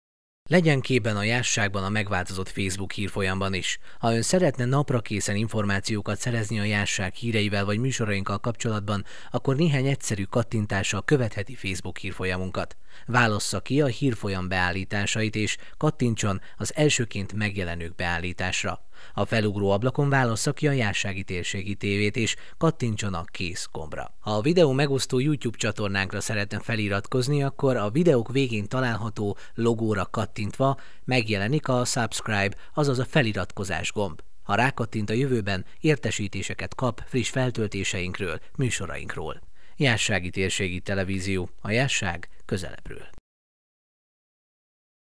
HOGYAN KÖVETHETI A JÁSZSÁGI TÉRSÉGI TV HÍREIT? MUTATJUK!
Legyen képben a Jászságban a megváltozott a facebook hírfolyamban is. Ha Ön szeretne naprakészen információkat szerezni a Jászság híreivel, vagy műsorainkkal kapcsolatban, akkor néhány egyszerű kattintással követheti fb hírfolyamunkat. Válassza ki a hírfolyam beállításait és kattintson az elsőként megjelenők beállítására. A felugró ablakon válassza ki Jászsági Térségi Tv-t és kattintson a KÉSZ gombra. Ha rákattint, a jövőben értesítést kap friss feltöltéseinkről, műsorainkról. Jászsági Térségi Tv – A Jászság közelebbről!